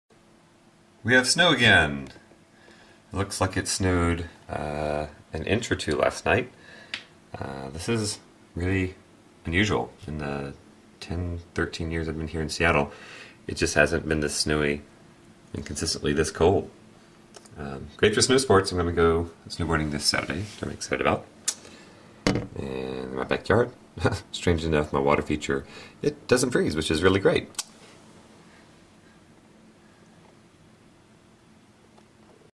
16 January 2007 snow in Seattle
Snow again. Wow, its a snowy cold year in Seattle, WA.